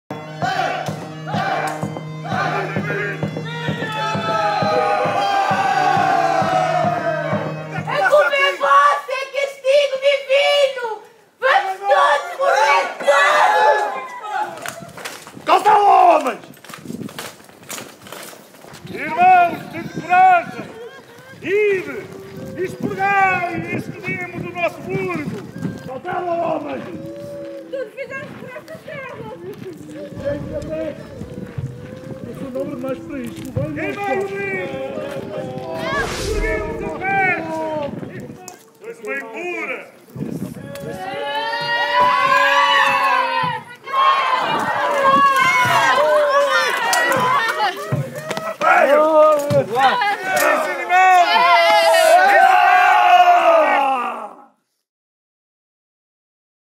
Ad Purgandum Obidos
No dia 1 Agosto, na Cerca do Castelo, em Óbidos, a empresa municipal Óbidos Criativa realizou uma iniciativa que teve como intenção «purificar Óbidos e afastar, de vez, a peste que assola este território».
Tratou-se de uma acção teatral, no âmbito do Mercado Medieval de Óbidos, que decorreu sem espectadores, e que será transmitida, posteriormente, para o grande público.